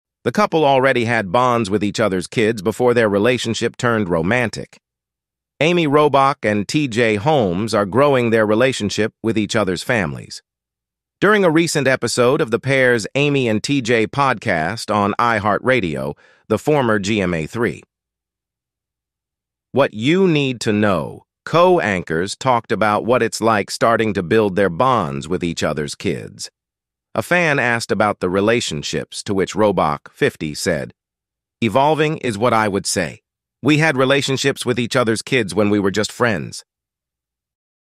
Amy Robach and T.J. Holmes Say Relationships with Each Other's Kids Are 'Evolving'
Amy Robach and T.J. Holmes Say Relationships with
Each Other's Kids Are 'Evolving'
: 'Been Very Thoughtful'